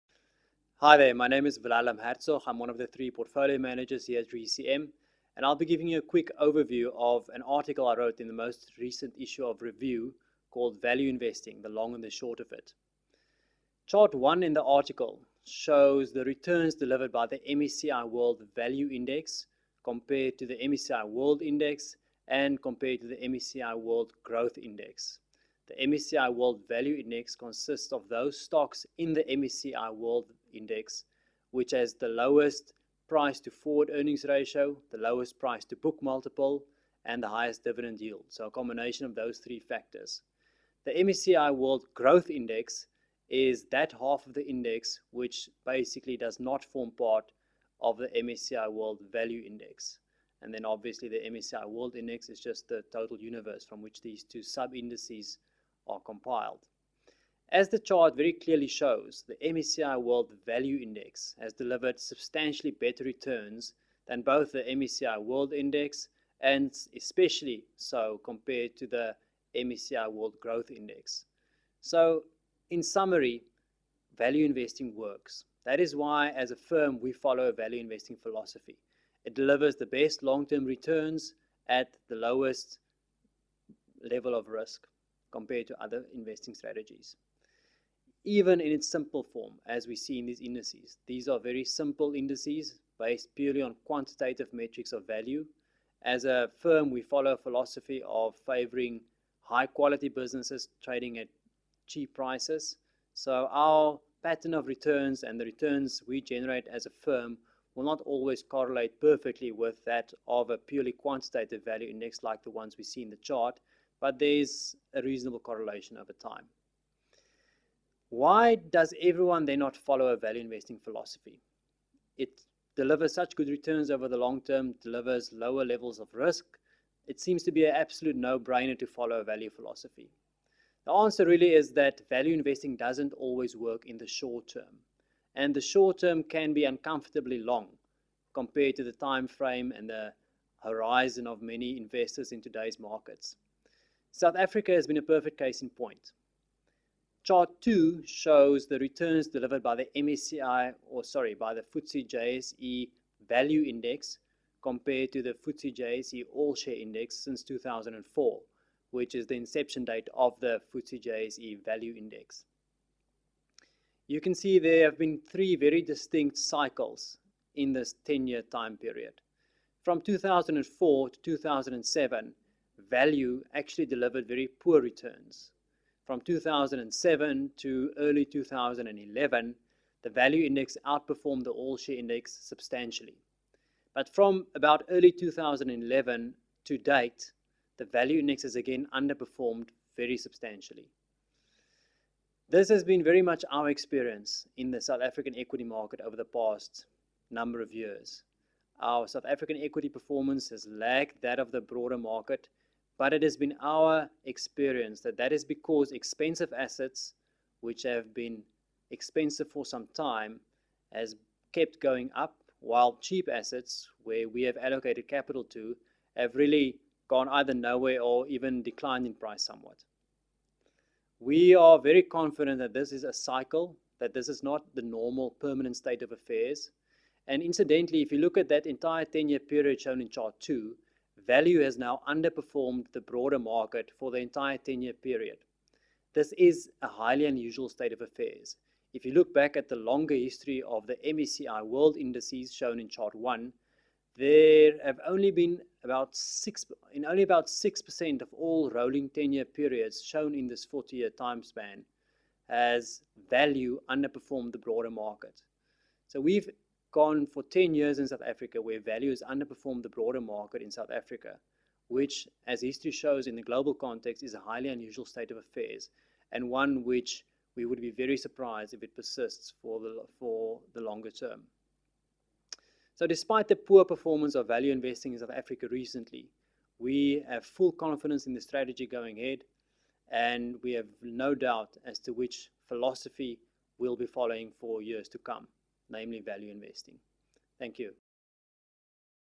Wilhelm Hertzog, RECM Portfolio Manager: Value Investing - The Long and the Short of It
Wilhelm questions why, given the clear evidence that value investing outperforms over time, most investors still don’t follow this strategy. He analyses value’s performance in South Africa over the last 20 years and explains why this is a highly unusual state of affairs in the global context. He discusses how this trend is currently playing out in the local market and why this still doesn’t inspire RECM to change its approach.

This video does not constitute financial advice. for more information.